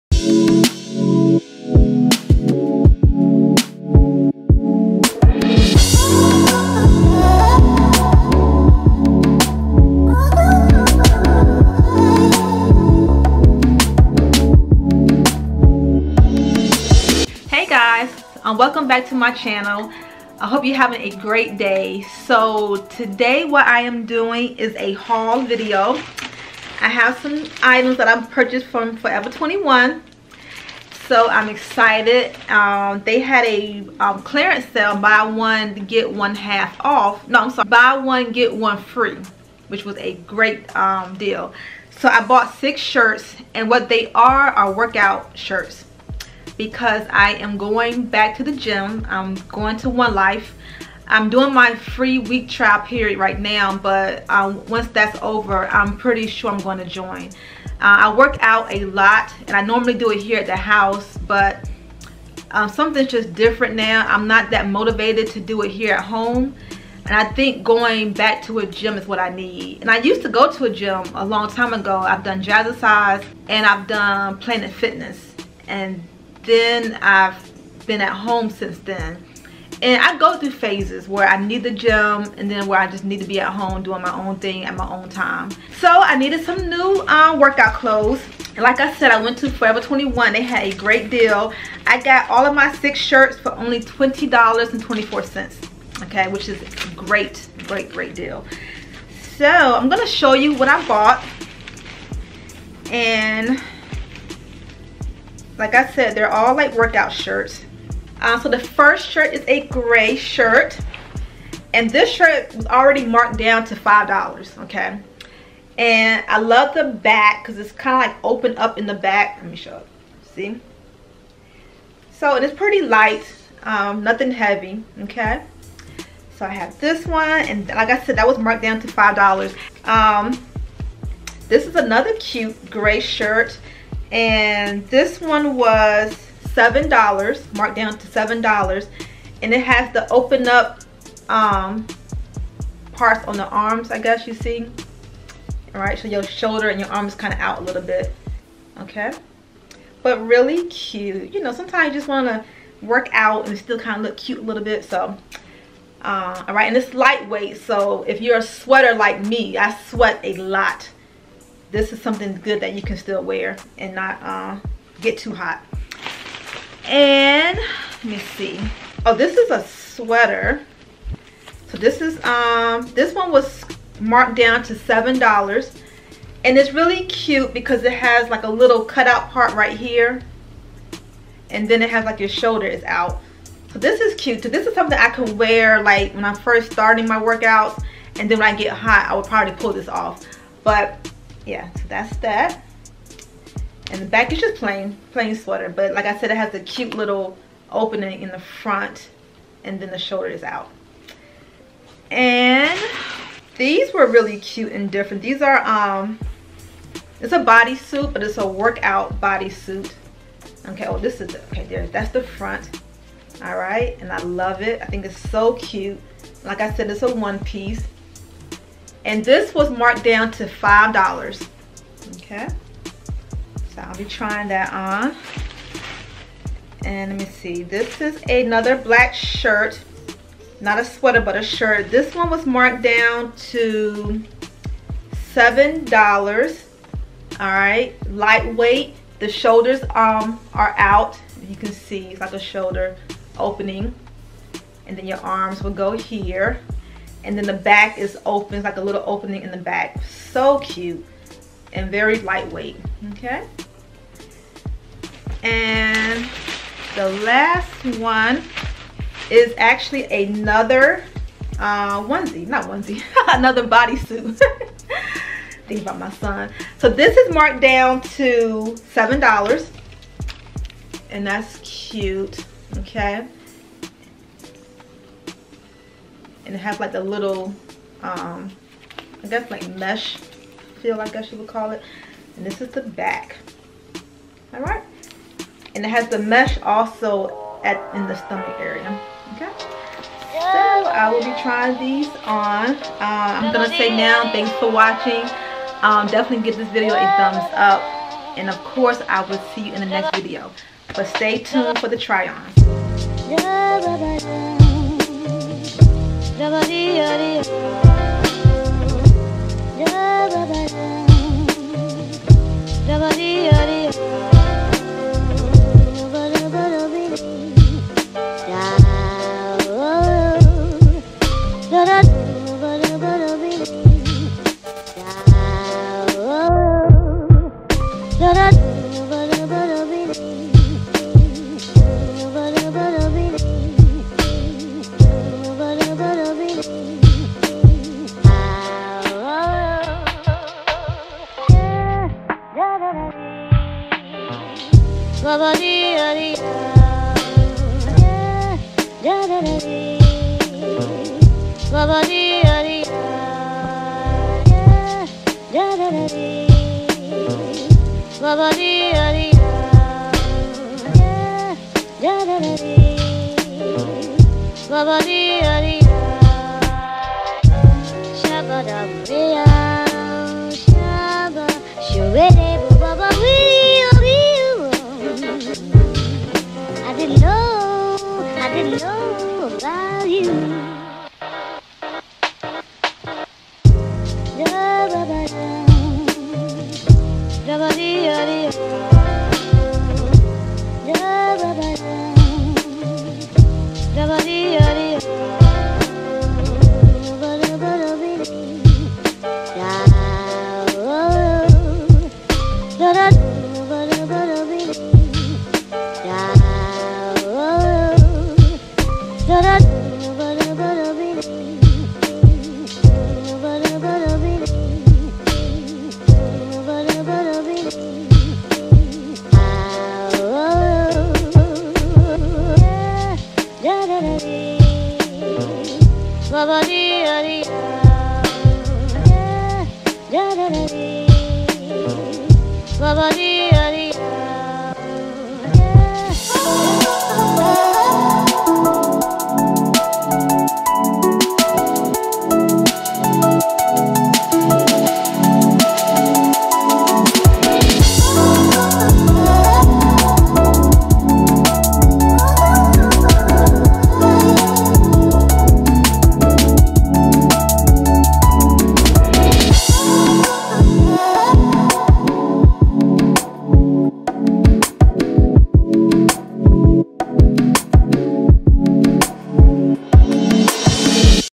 FOREVER 21 HAUL | AFFORDABLE CUTE WORKOUT CLOTHES| ACTIVE WEAR
Hey I got some great deals at Forever 21! I needed some new workout clothes b/c I'm joining One Life Fitness Center. I bought 6 shirts for $20.24. And they are so cute!!